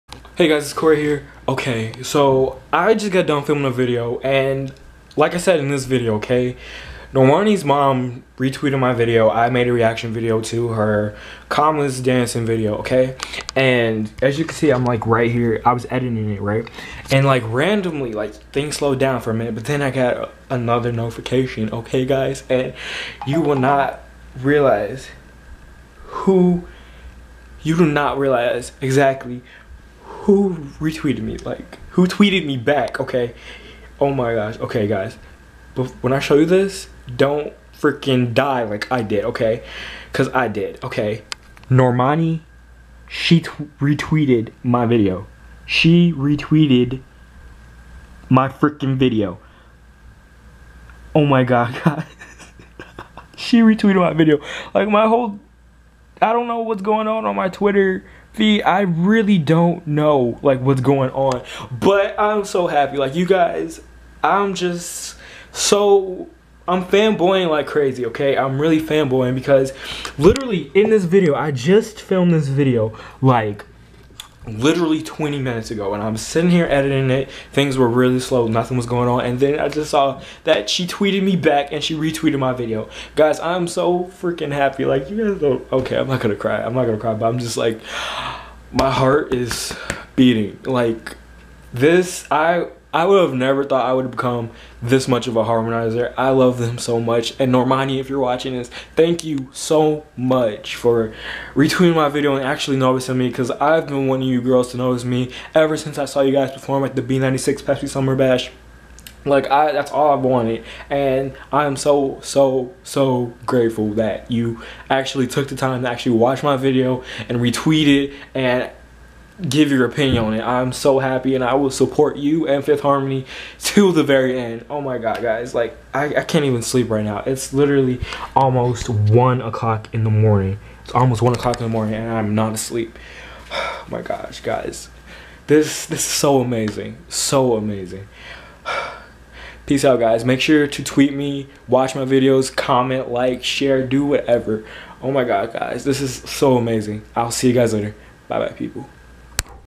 NORMANI (FIFTH HARMONY) TWEETED ME & MY VIDEO! (REACTION + VLOG)
I HAD SO MANY EMOTIONS GOING THROUGH MY HEAD WHEN SHE TWEETED ME! MY HEART LITERALLY FELT LIKE EXPLODING THROUGH MY CHEST!
THANK YOU SO MUCH NORMANI!